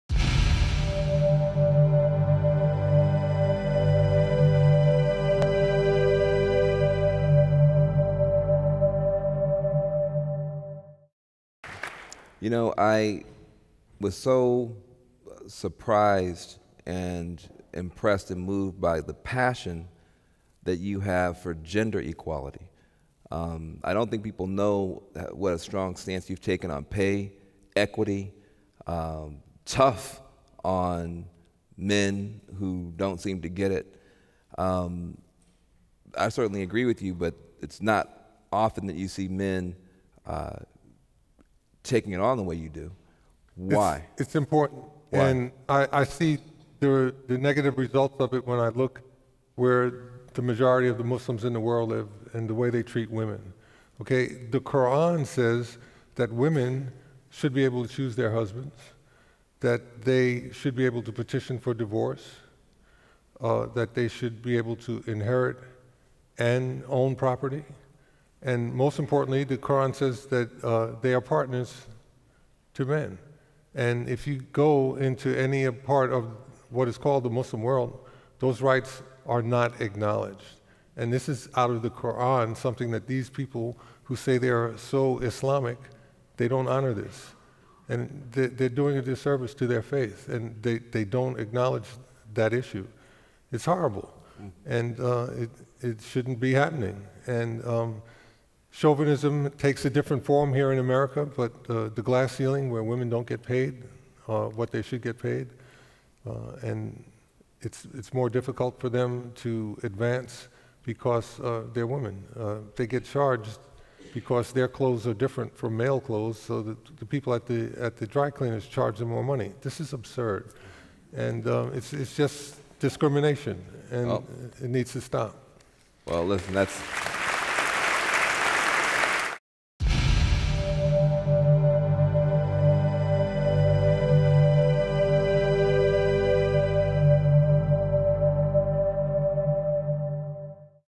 An Evening with Kareem Abdul-Jabbar (Clip 5: Gender Equality)
An American Icon Lets Loose on Politics, Race, Religion, Inequality, Sports, and Media

Kareem Abdul-Jabbar, NBA Hall of Fame Inductee; Columnist, Time Magazine; Author, Writings on the Wall: Searching for a New Equality Beyond Black and White

In conversation with Van Jones, CNN Commentator

Since retiring from professional basketball as the NBA's all-time leading scorer, Kareem Abdul-Jabbar has become a lauded observer of culture and society, a New York Times bestselling author, and a regular contributor to The Washington Post, Time magazine and Time.com. At a pivotal time in our history and in the midst of a presidential race that threatens to divide us, Mr. Abdul-Jabbar will give his take on the origins of bias and inequality that remain a stubborn part of America, 240 years after its founding document declared that all Americans are created equal. He contends that many Americans, out of fear and sometimes ignorance, make too many false assumptions about fellow citizens who aren’t like them.

Join Mr. Abdul-Jabbar for an unabashed and important conversation on how America can be a more unified country.